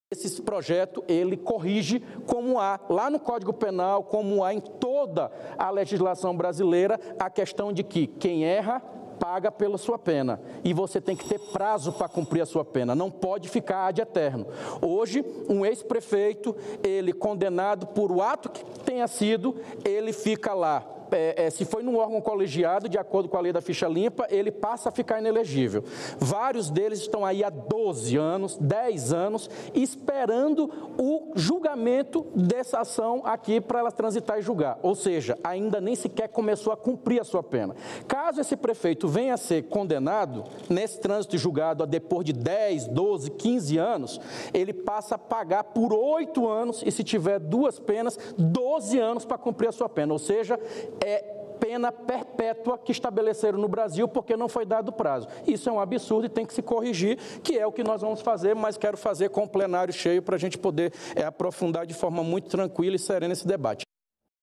Projeto que altera Lei da Ficha Limpa passa a ser analisado em regime de urgência
O Plenário aprovou requerimento de urgência para análise da proposta que altera a Lei da Ficha Limpa (PLP 192/2023). O projeto define apenas um período de oito anos em que o político fica impedido de se candidatar. O prazo de inelegibilidade deve passar a contar a partir da data da condenação.